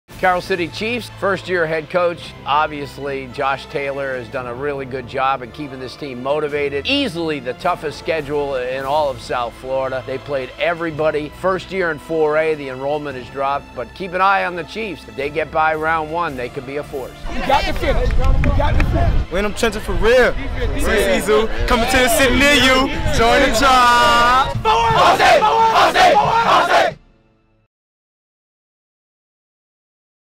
Miami Carol City Football
- Presents Pursuit '19
- Larry Blustein analyzes the Miami Carol City Chiefs 2019 FHSAA playoff run
- South Florida High School Football